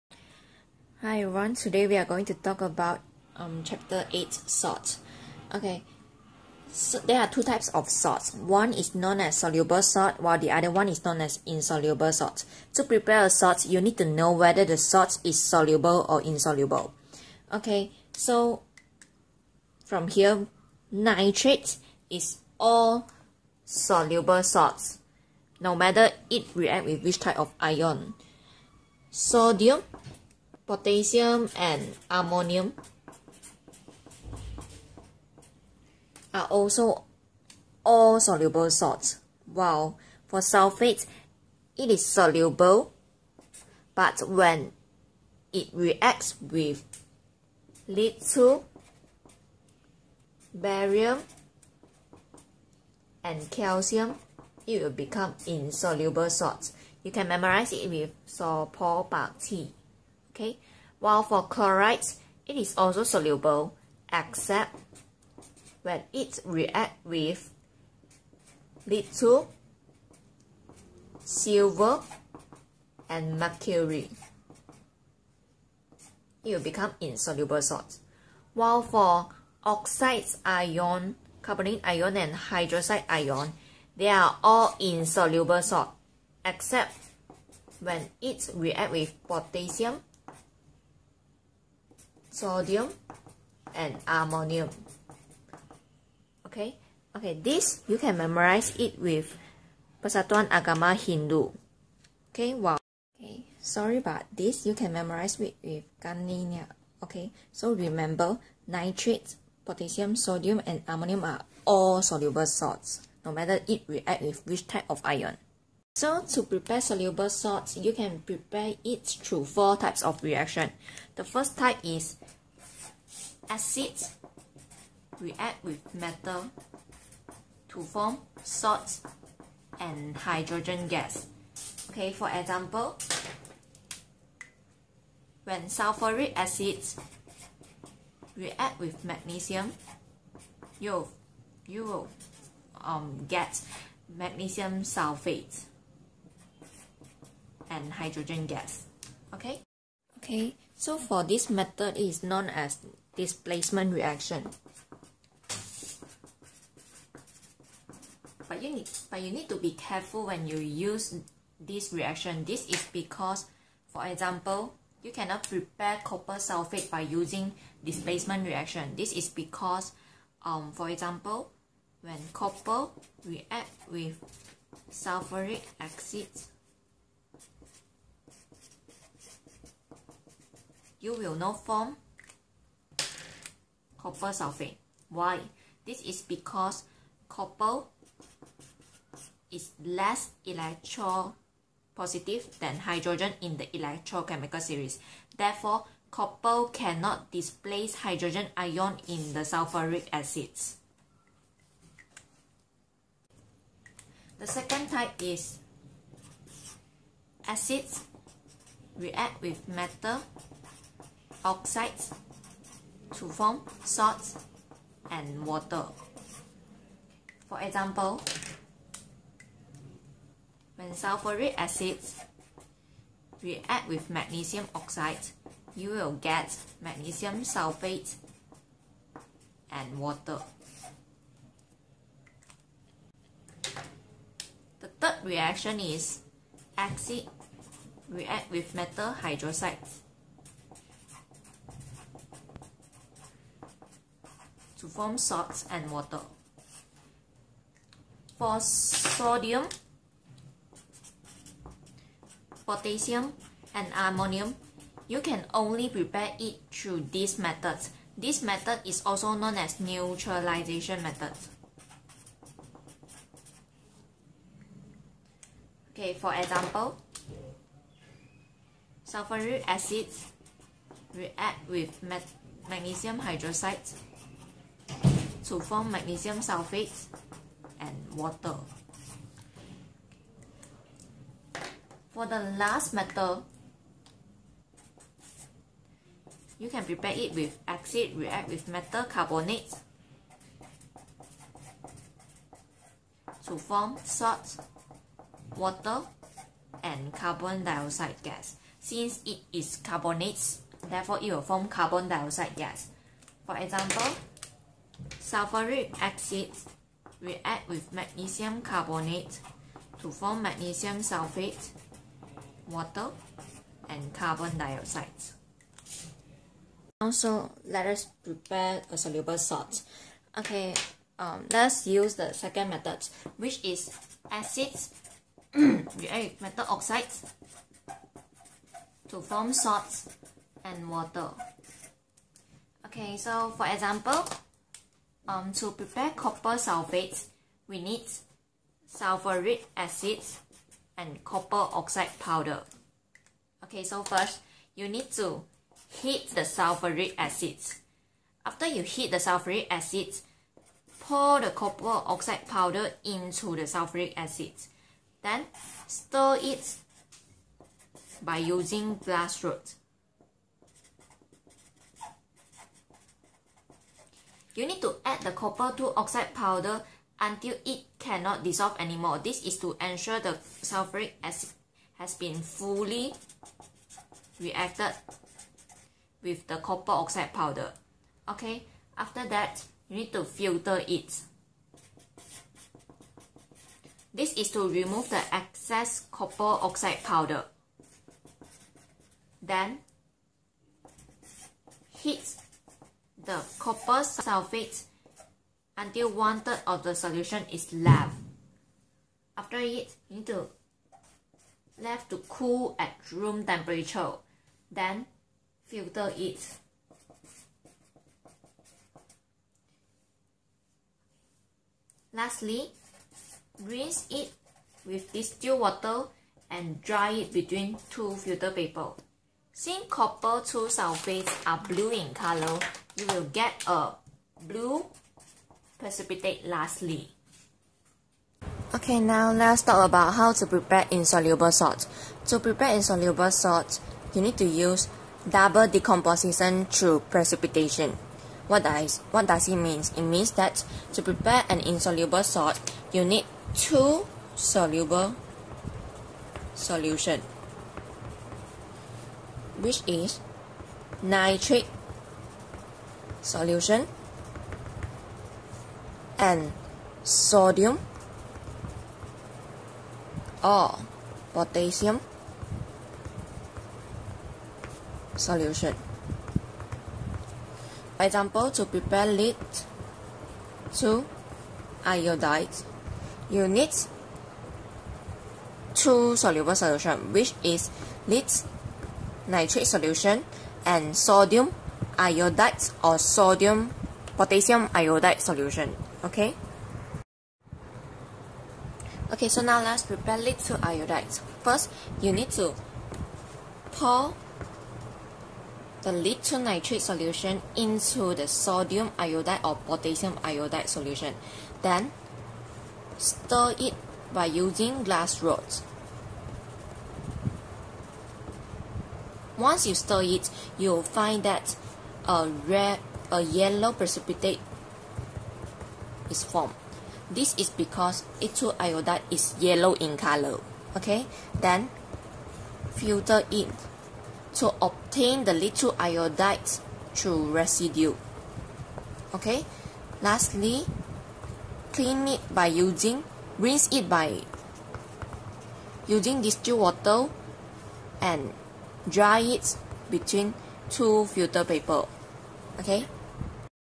Chemistry - Salt
Learn to prepare soluble salt and insoluble salt by Ms Huey Lee